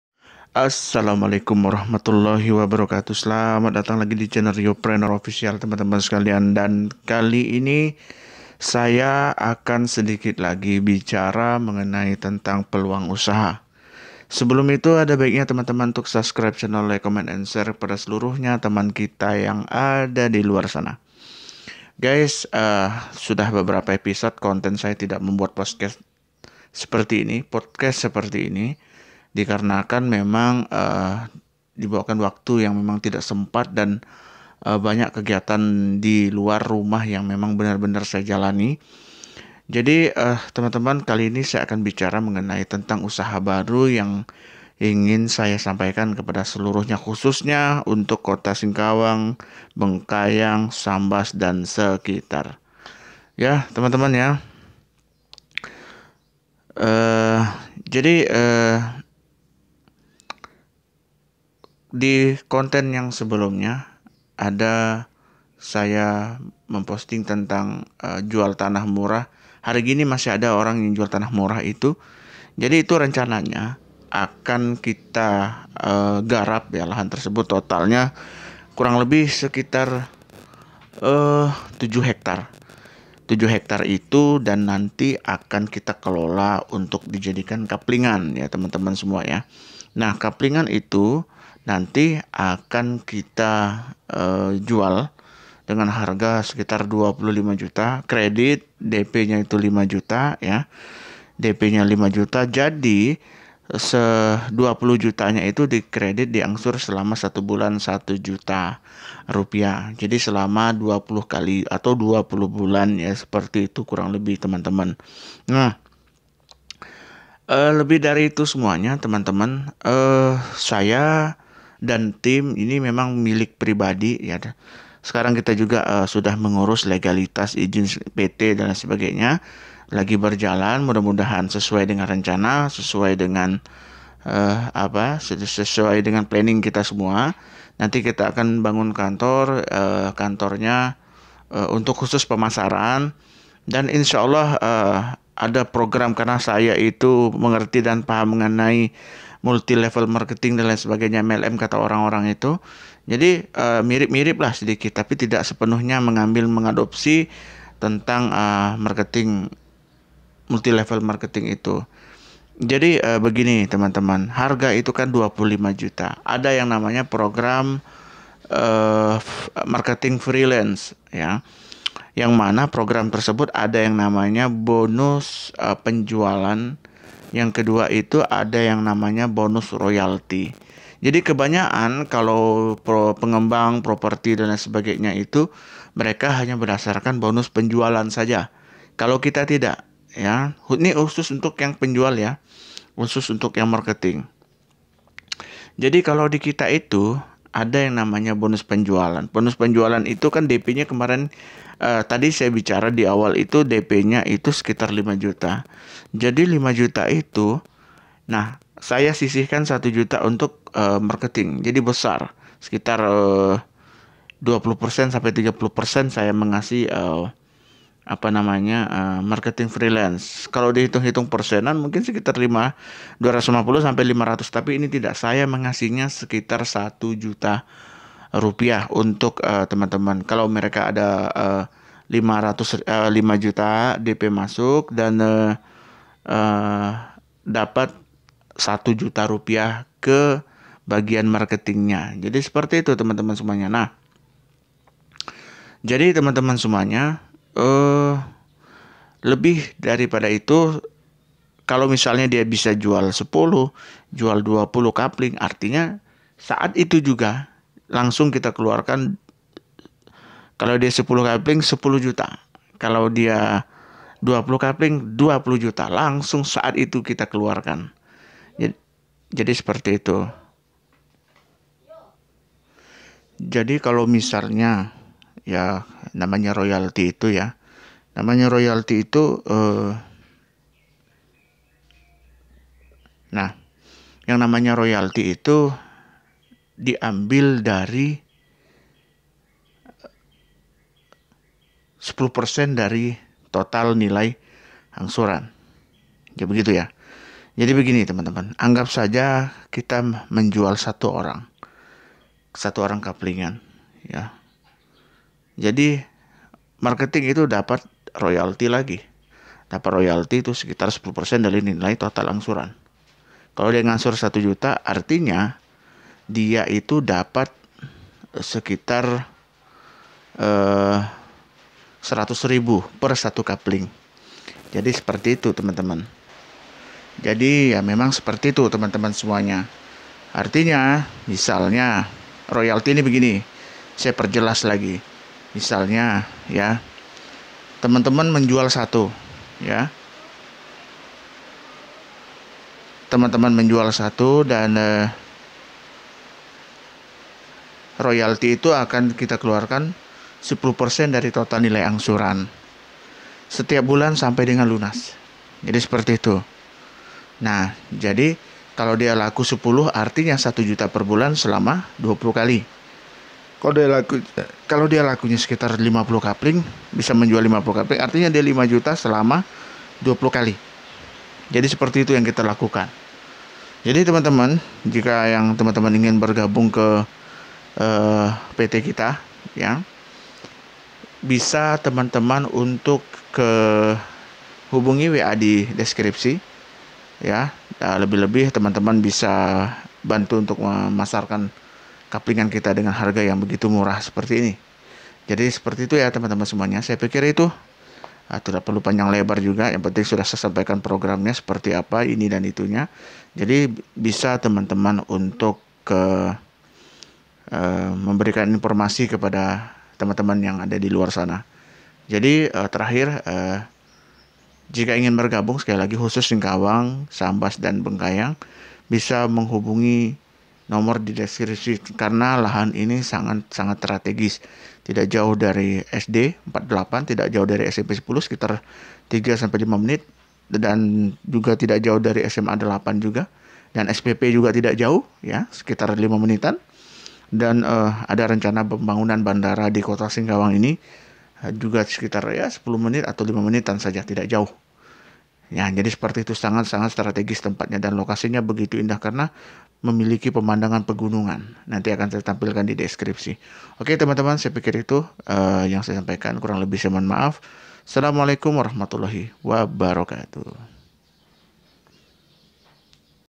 VLOGRIO - KAPLINGAN MURAH CUMA 25 JUTA UKURAN 10 X 20 KREDIT 2 TAHUN DP 5 JUTA ||| KOTA SINGKAWANG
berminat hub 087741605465